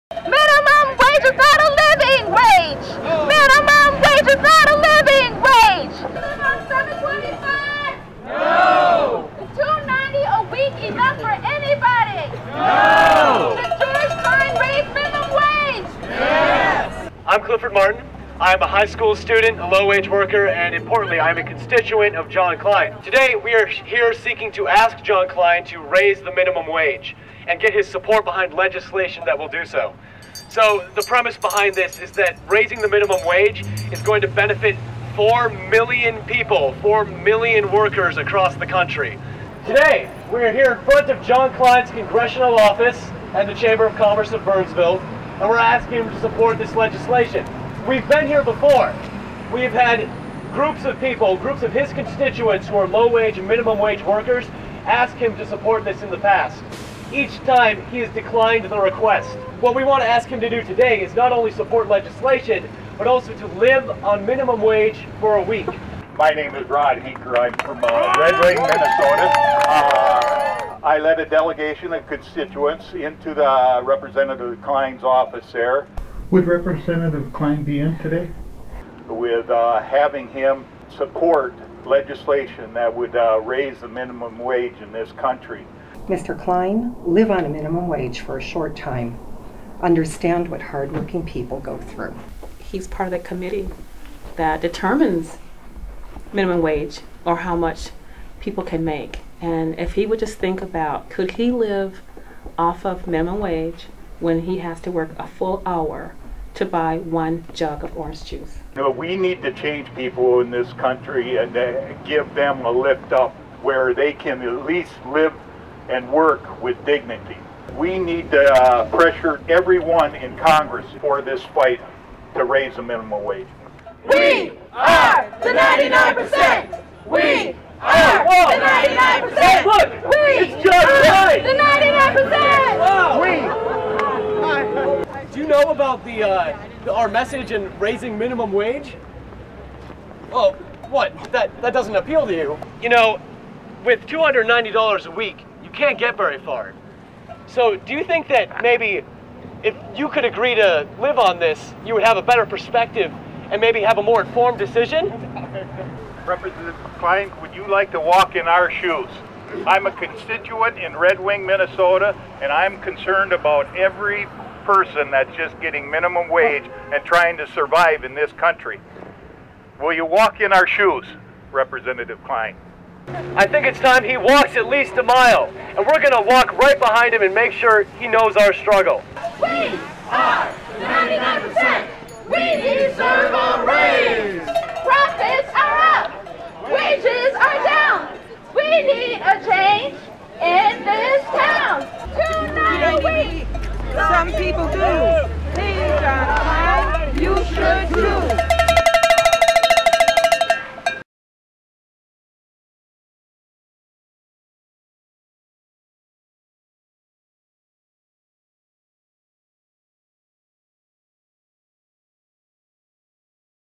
Constituents to Rep. John Kline: Minimum Wage is Not a Living Wage
Raising the minimum wage is an important step toward an economy that works for everyone, not just the richest 1%. That's why both current elected officials and those who are pursuing elected office need to stand with us in raising the federal minimum wage.

Time and again, U.S. Representative John Kline (MN-02) has stood in the way of an increase in the minimum wage. On Tuesday, July 24, we gathered at his office in Burnsville and called on him to "Live Like the Rest of Us."

A Minnesotan working 40 hours a week at minimum wage grosses about $290 a week and is forced to live on that amount. We asked Rep. Kline to do the same. How far does he think $290 goes?